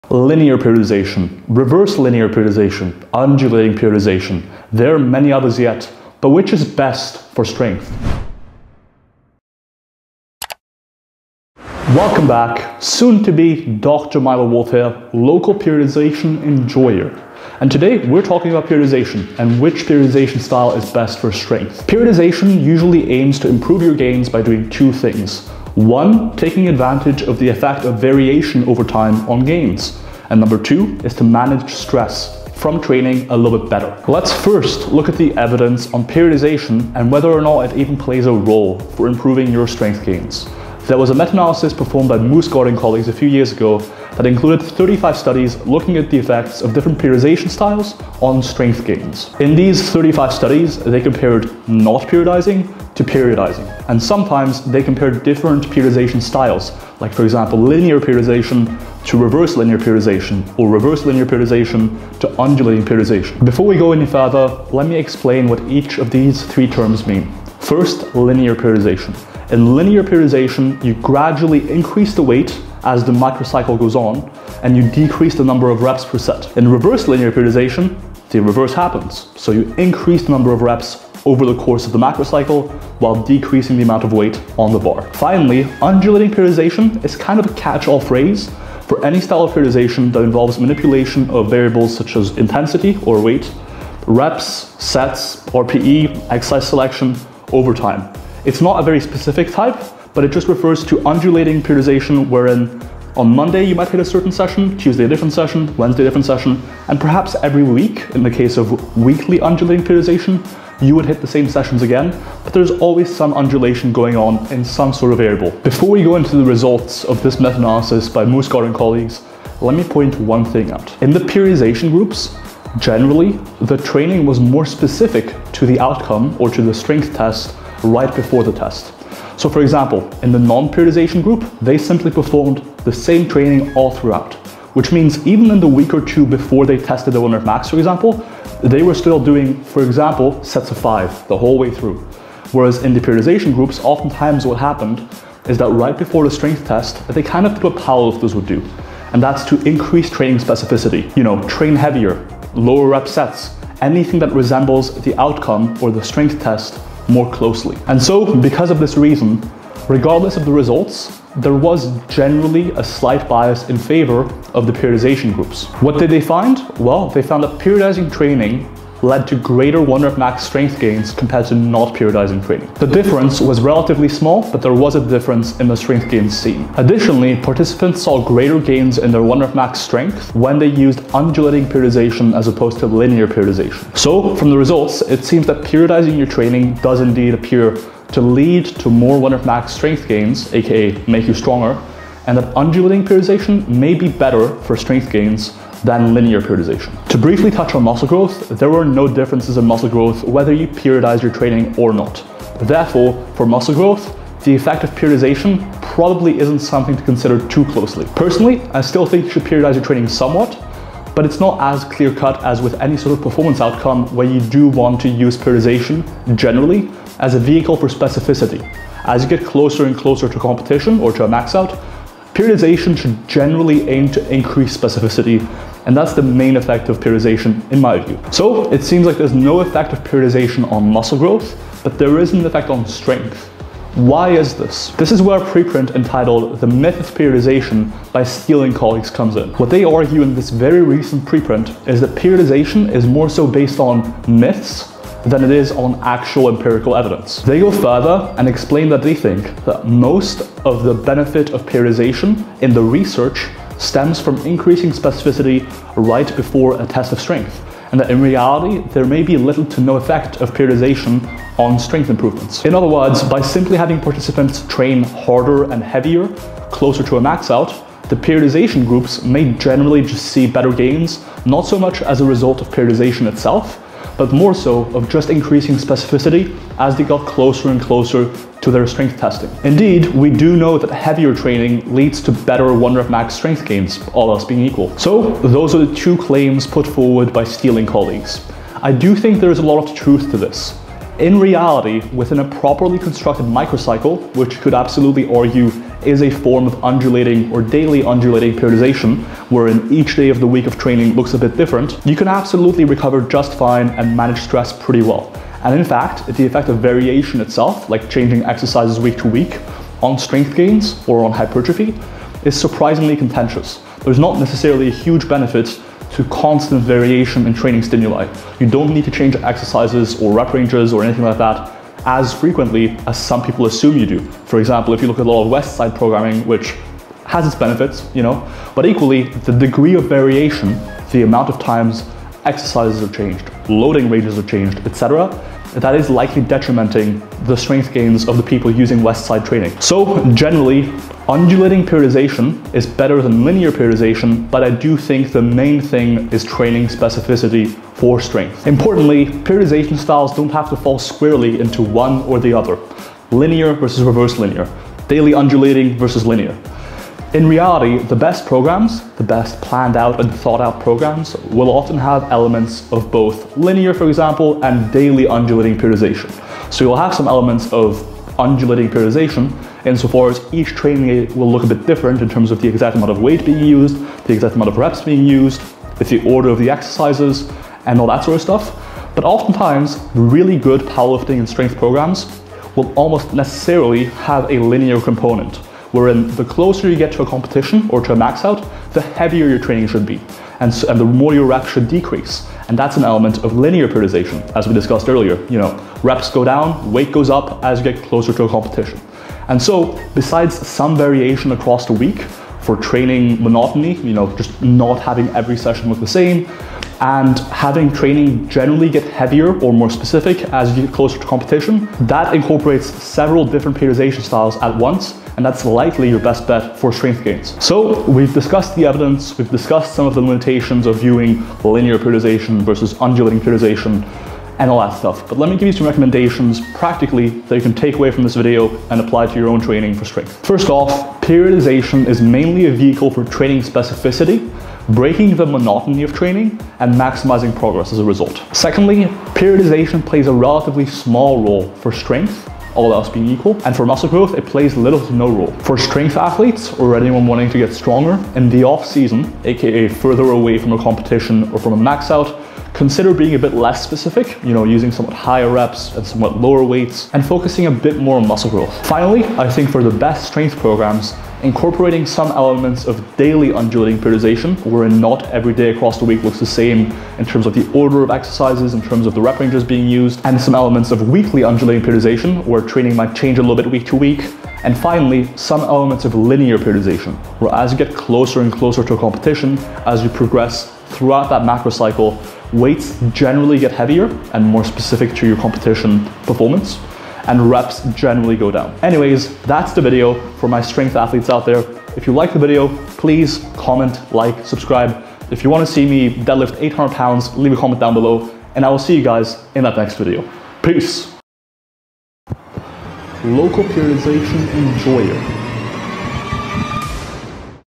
What's the BEST periodization style for strength? (Science Explained)
In this video we discuss periodization in the context of strength, looking at the available data to come to some conclusions as to what the best style is, and how to implement it into your own training.

Timestamps:
00:00 - 01:02 (Intro)
01:03 - 01:54 (Linear vs non-linaer vs undulating)
01:55 - 02:45 (A slight caveat)
02:45 - 03:53 (The results)
03:54 - 06:56 ("The myth of periodization")
06:57 - 08:07 (Practical recommendations)
08:08 - 08:28 (Outro)

"What is the BEST periodization style for strength? (Science Explained)"